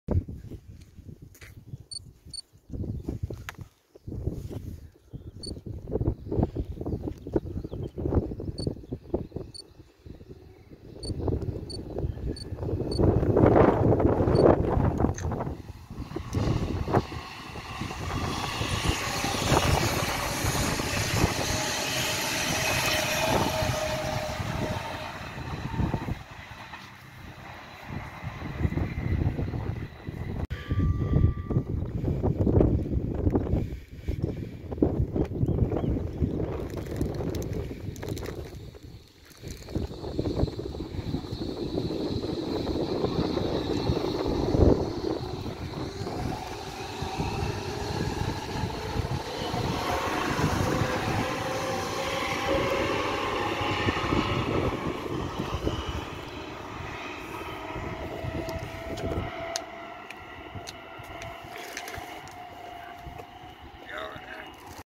S-Bahn Berlin: Zwei S-Bahnen der S8 in Schönfließ BR 480
Aufgenommen am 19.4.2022. Mit dabei waren: @nahverkehrdusseldorfundumg8364. Sein Instagram: nahverkehr_duesseldorf und @busbahnfan6157. Sein Instagram: bus_bahn_fan.